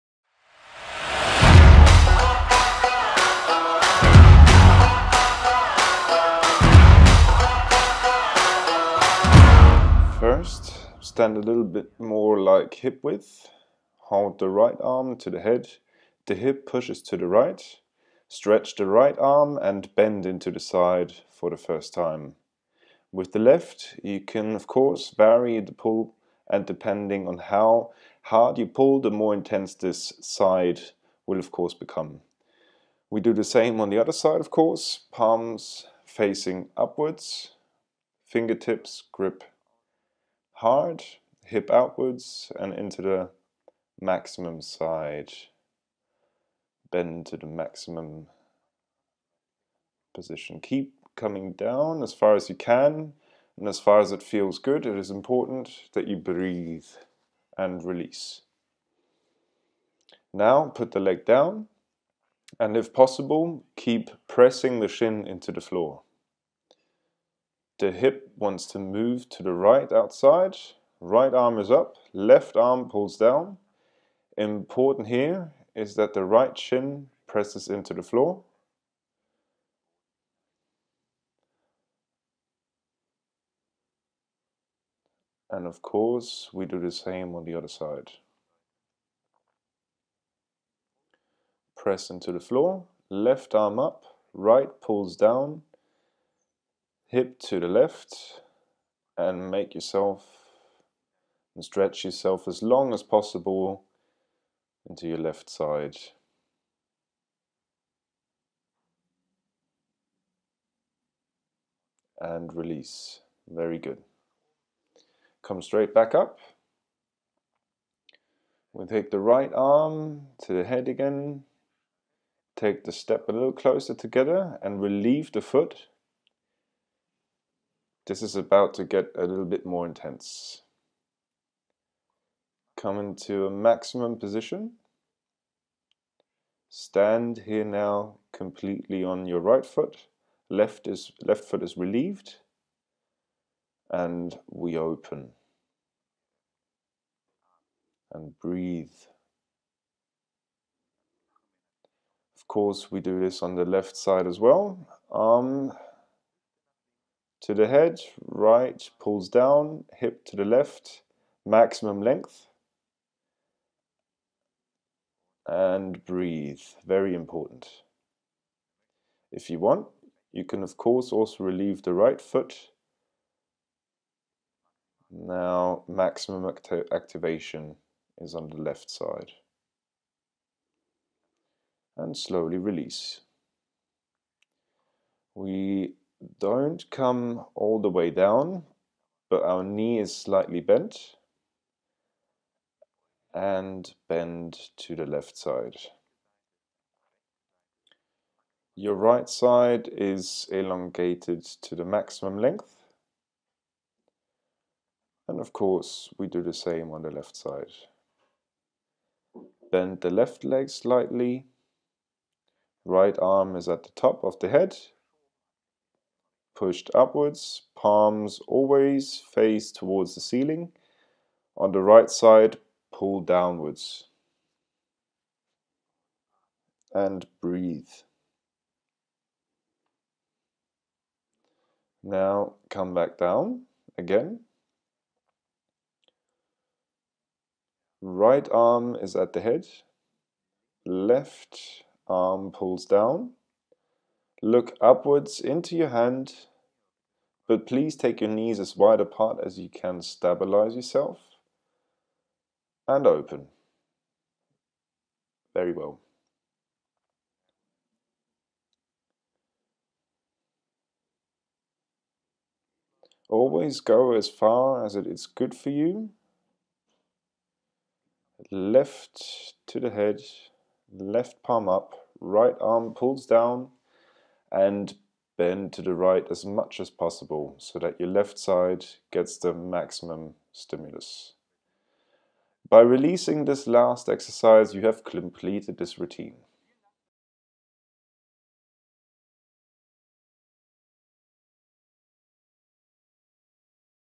5 MIN mobility exercises for the lateral fascia chains | Muscle length training | BLACKROLL®
Do you do endurance sports or breathe heavily? These muscle length training exercises for the lateral fascia chains will give you more length in your muscles after sporting or one-sided activities. You become more flexible and can prevent tension.

⌚️ 5 minutes

✅ Release tension
✅ Adjust dysbalances
✅ Improve mobility
✅ Prevent pain due to one-sided activities
✅ Best for endurance athletes and people who breathe heavily
✅ short, simple & effective

The lateral muscle-fascia chain runs from the bottom of the foot on the side of the body up to your skull bone. Its postural function is to balance our front and back of the body. Due to our one-sided activities and some sporting activities, it often tends to shorten. The respiratory muscles are also part of this chain. The exercises in this routine are therefore also suitable for all endurance athletes or people who breathe heavily.

Pay attention to the following during muscle length training:
👉 The flexibility exercises are held for 20-25 seconds or 5 breaths.
👉 Breathe in & out deeply and do not hold your breath under any circumstances.
👉 Listen to your body and adjust the exercise intensity & pace accordingly.

Enjoy your muscle length workout & cheers to your flexibility. 🙌

P.S. You don't have to be mobile to do muscle length training. The exercises are designed so that everyone can participate. 😊 Better flexibility comes with routine. We recommend you spend 15 minutes a day with the STRETCH BAND and incorporate flexibility exercises into your daily routine. 😇